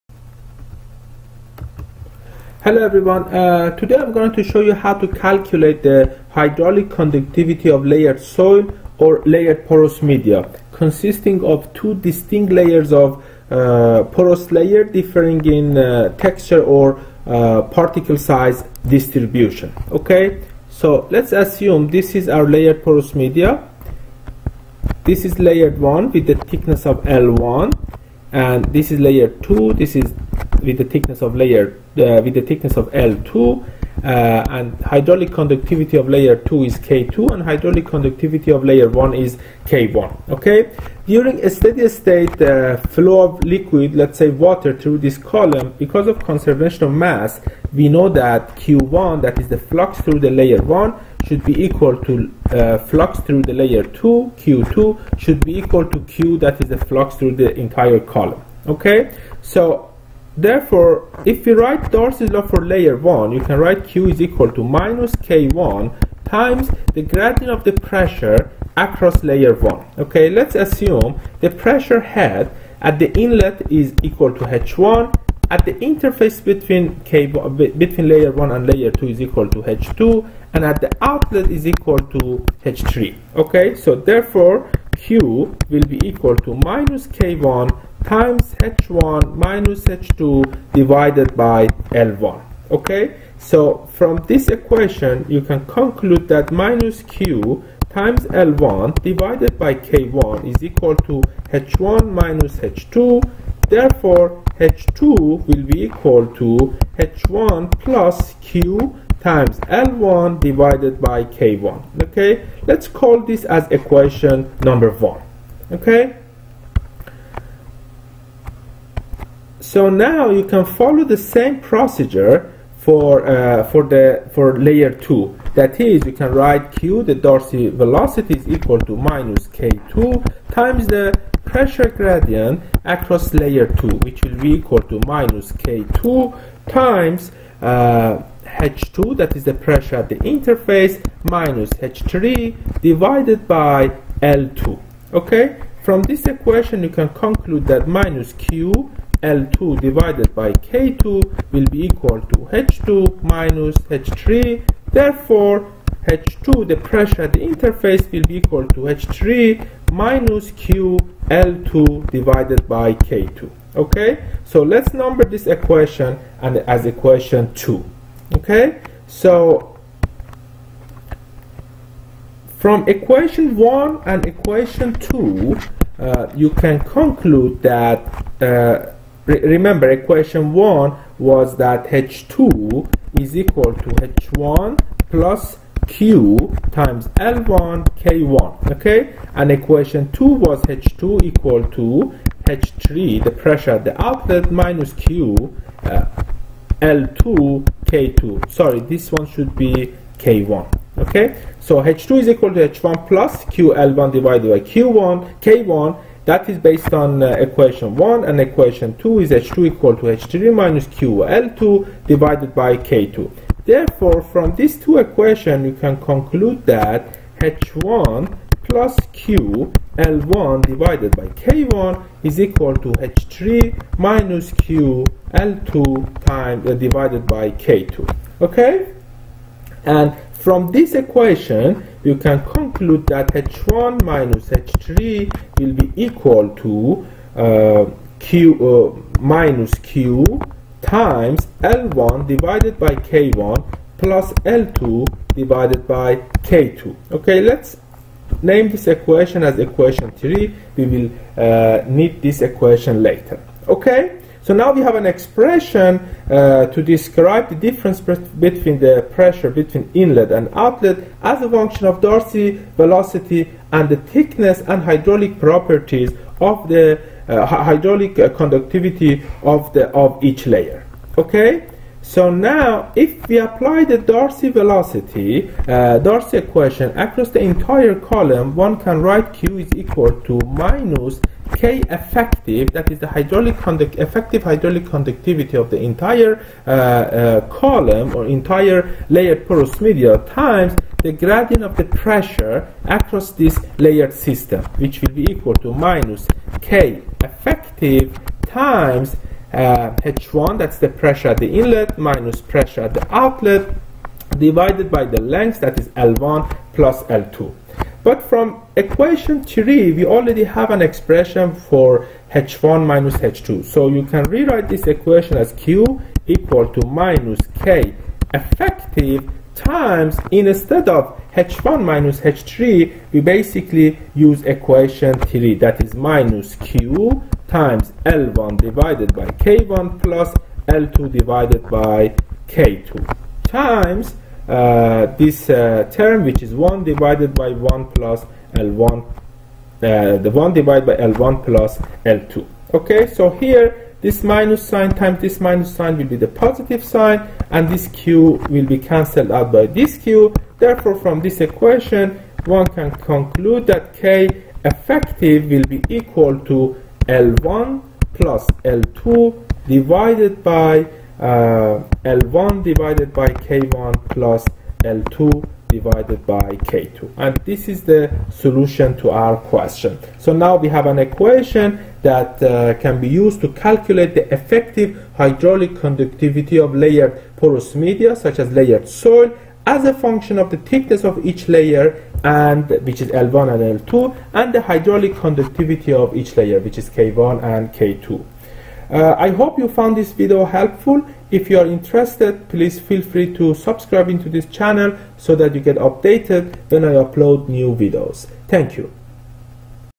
Hydraulic conductivity of layered soil
This video shows how to calculate the effective saturated hydraulic conductivity of layered porous media such as soil as a function of the thickness and saturated hydraulic conductivity of each layer.